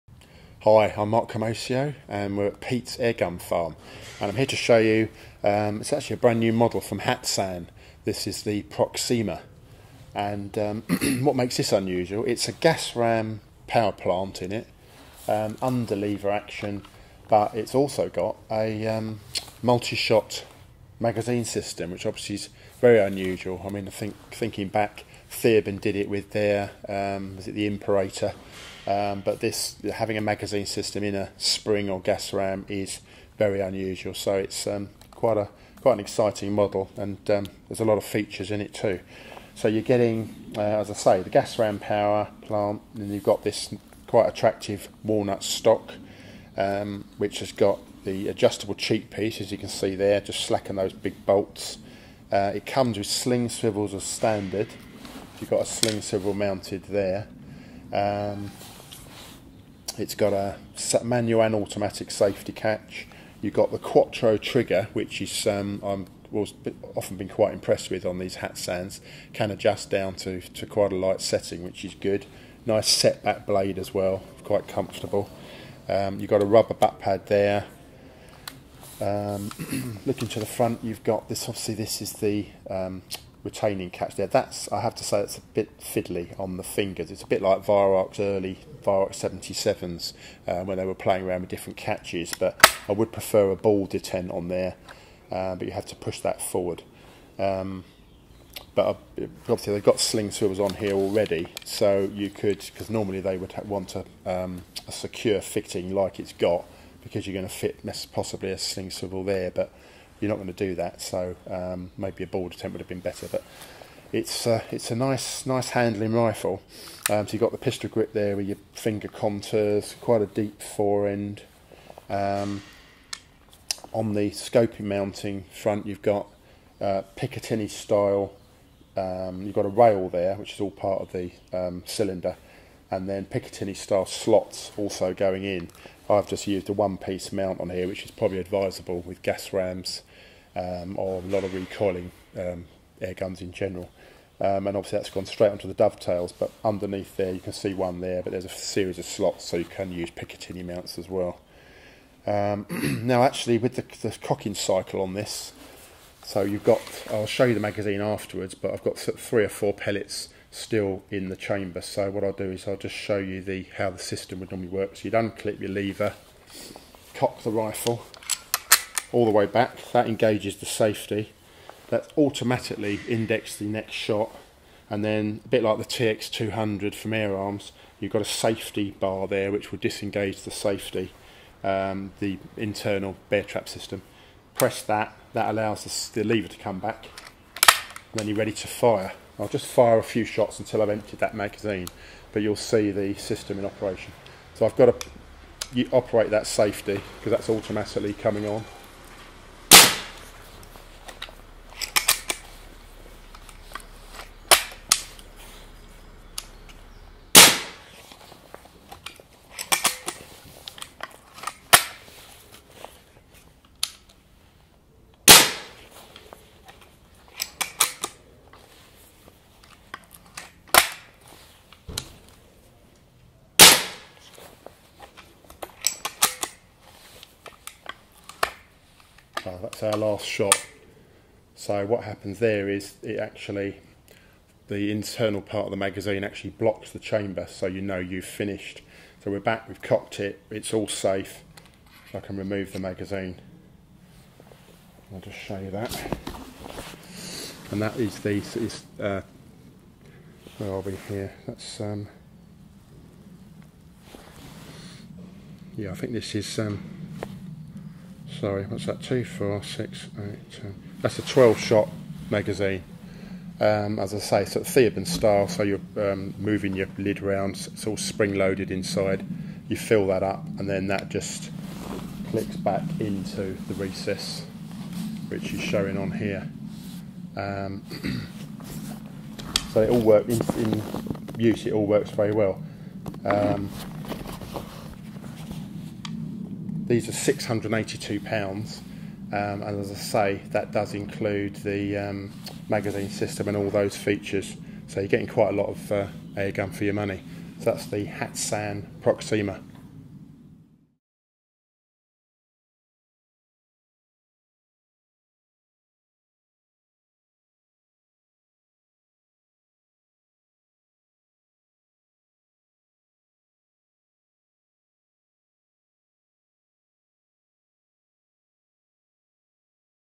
Hatsan Proxima Gas Ram Underlever Multishot Airgun Review
Mark Camoccio takes a look at a pretty unusual Hatsan, the Proxima.